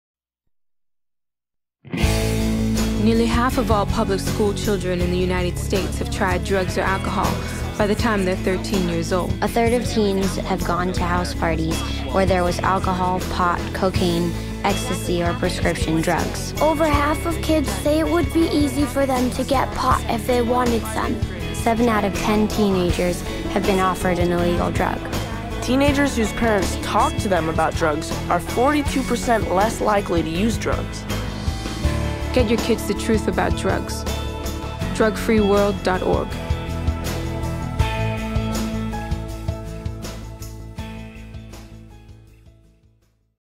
Sannheten om stoff
Det anslås at det er 208 millioner mennesker i verden som bruker ulovlige stoffer. Sannferdig informasjon om stoffmisbruk kan redde menneskeliv -- en offentlig informasjonsfilm.